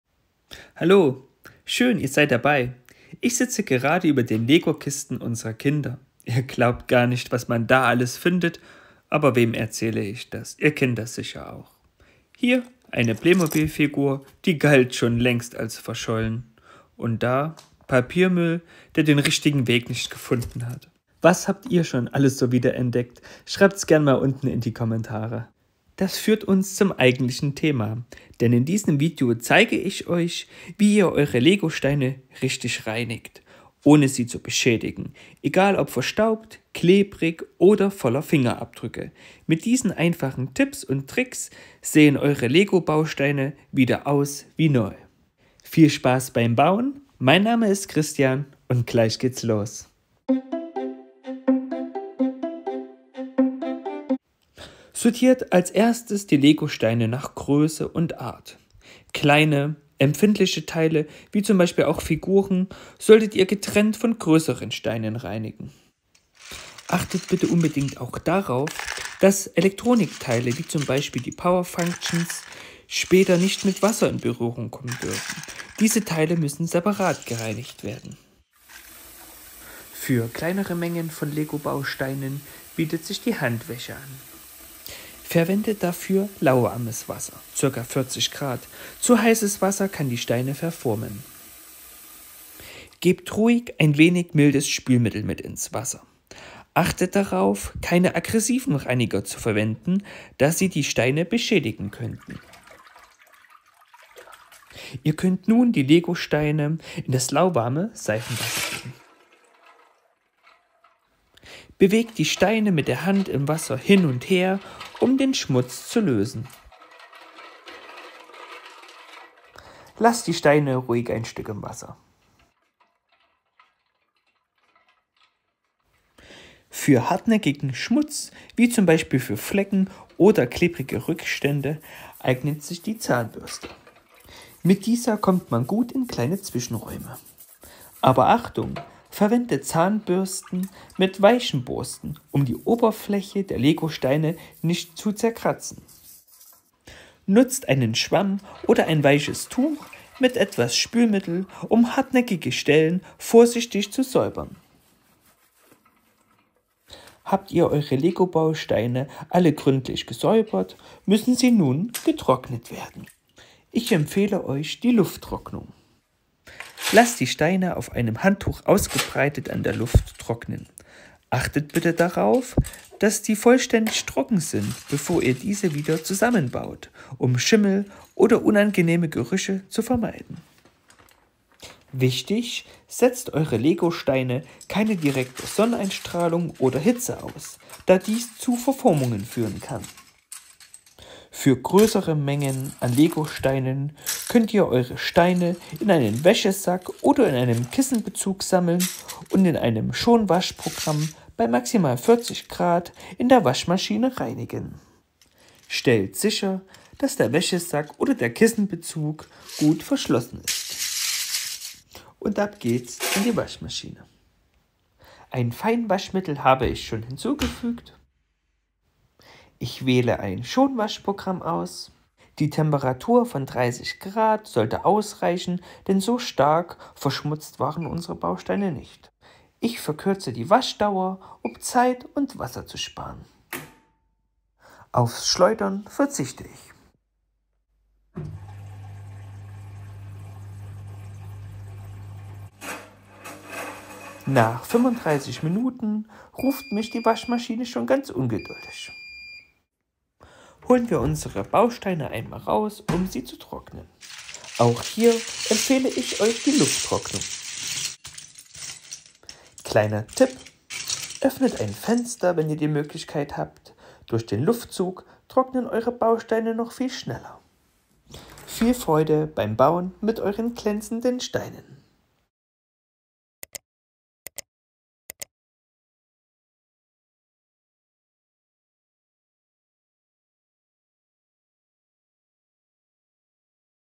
So reinigst du LEGO®-Steine richtig! 🧼 Tipps & Tricks für saubere Klemmbausteine
Die ultimative Anleitung: LEGO®-Steine richtig sauber machen! 🧽

Verstaubte und dreckige LEGO®-Steine gehören der Vergangenheit an! In diesem Video zeige ich euch, wie Ihr eure Steine schnell, effektiv und schonend reinigen könnt. Ob für eure Sammlung oder einfach für den
Spielspaß - nach diesen Tipps glänzen sie wieder wie am ersten Tag!

Was Du erfährst:

• Welches Reinigungsmittel für LEGO® sicher ist
• Wie Du auch größere Mengen LEGO® leicht reinigst
• Tipps, um LEGO® zu trocknen

Viele Grüße
Die Krallmanns